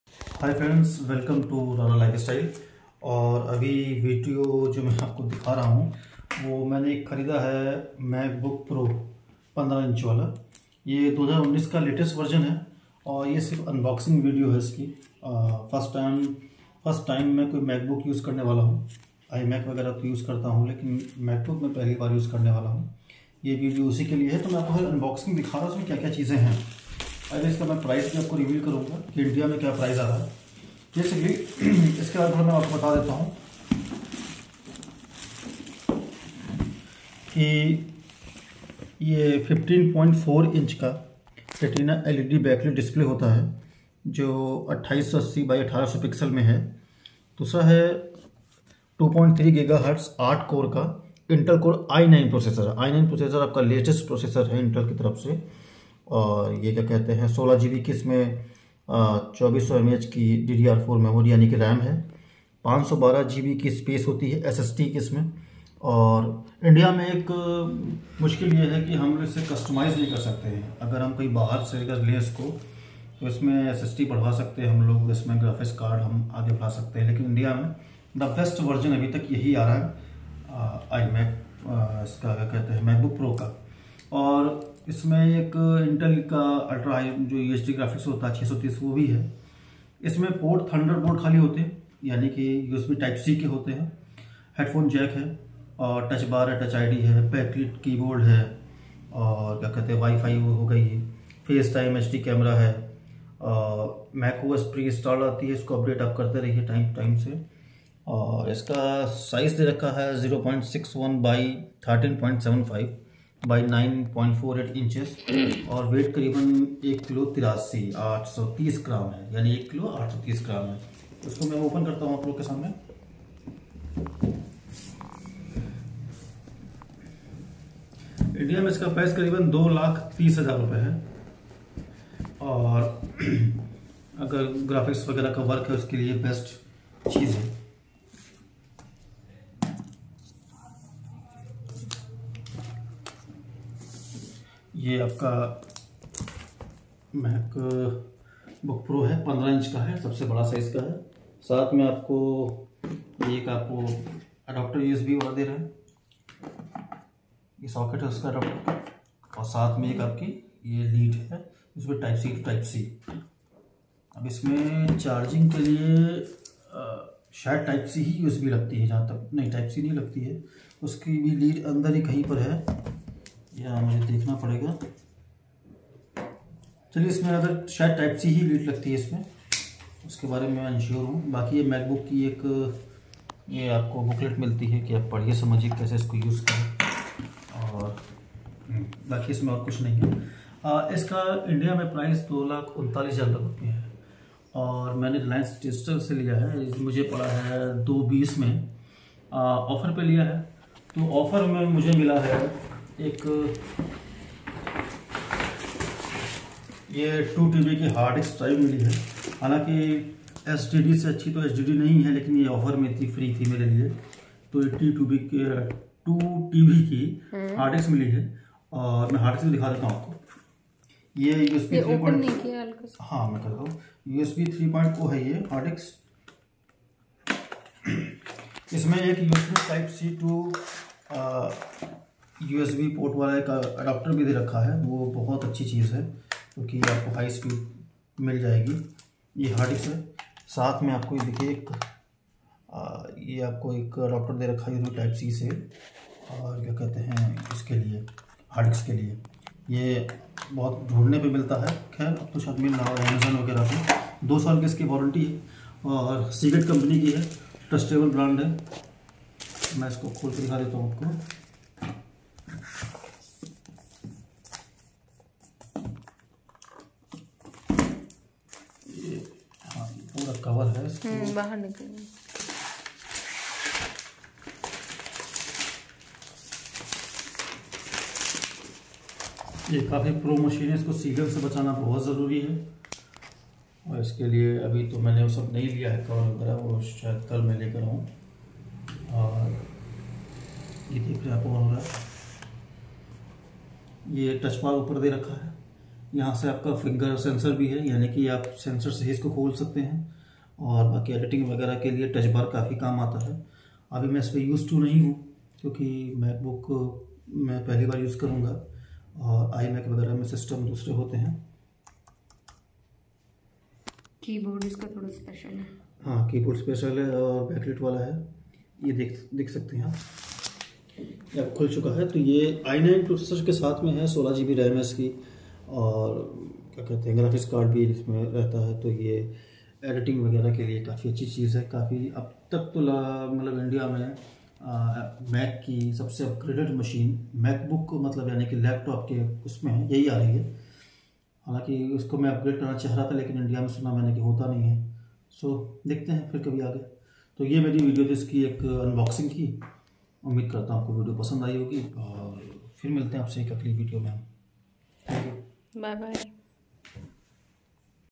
apple का लैपटॉप | Apple Mac Book Pro 15Inch Unboxing India | Price and other details |RARA LIFESTYLE
This is a unboxing video for apple mac book pro 15 Inch , brought from reliance_digital

meesho video

RARA lifestyle

Colorbar fruit drop hand cream
https: